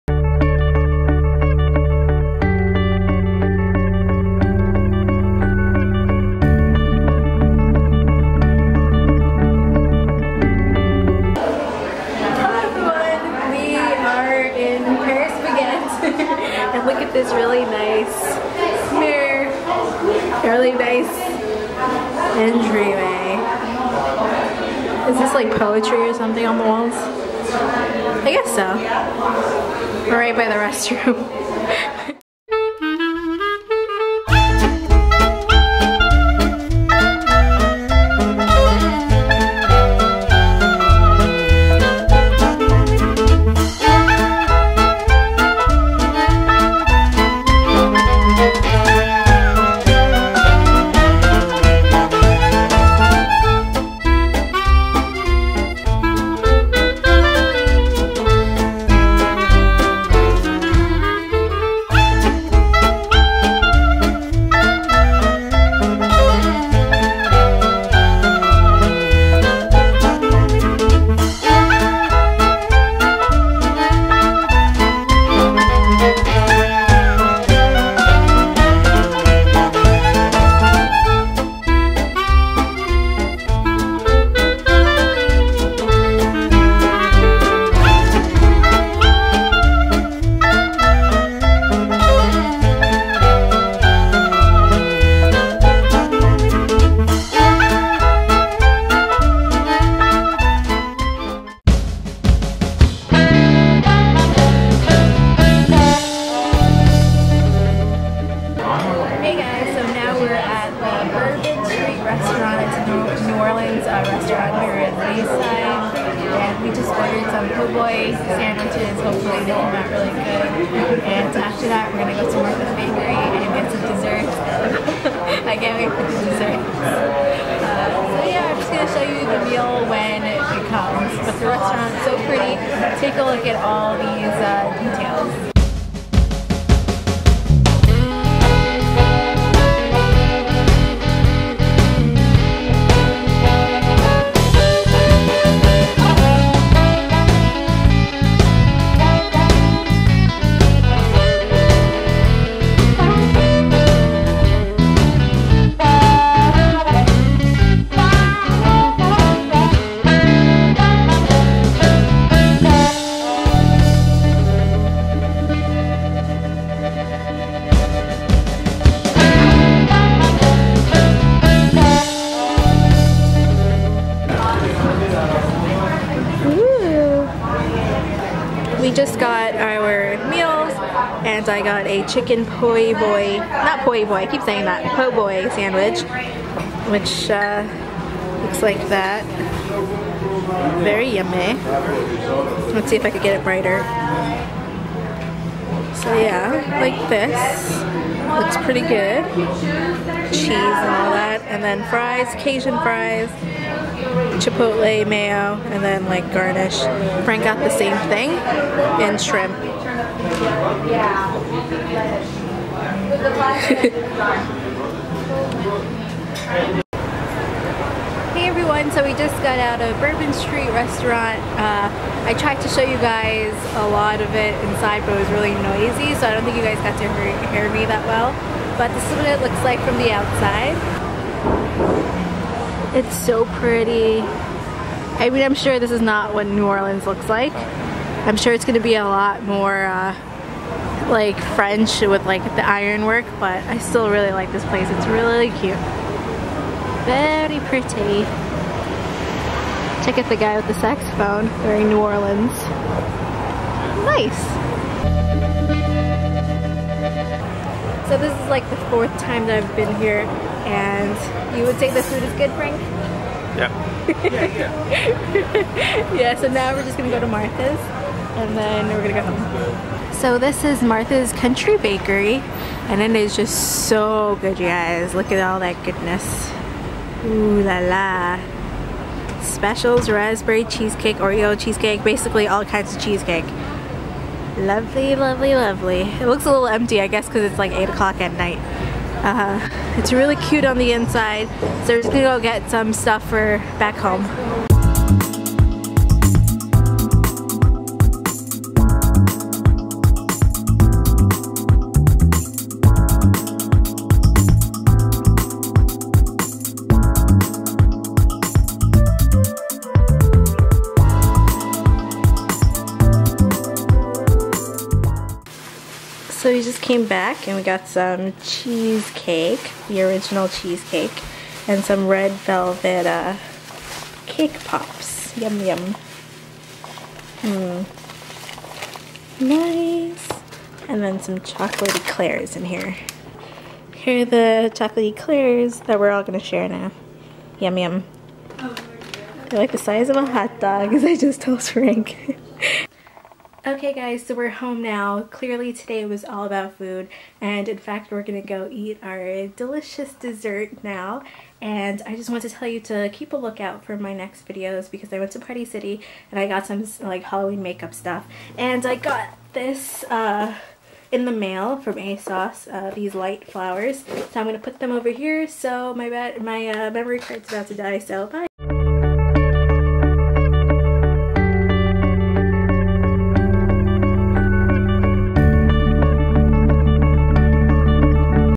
NYC Food VLOG: Bourbon, Paris Baguette and Martha's Bakery
Hey everyone! Here’s a vlog about Frank and I’s recent food adventure in my home of Queens, NYC. We had some delicious Parisian/Korean/mostly American? themed lunch. Next up, we had some good ol’ New Orleans food (gotta love that cajun!) and we finished the night by getting some sweet desserts for us and my family back home. Enjoy this quick video!

The places where we ate were: Paris Baguette (lunch), Bourbon Street Restaurant (Dinner) and Martha’s Country Bakery (dessert). They were all fairly near each other.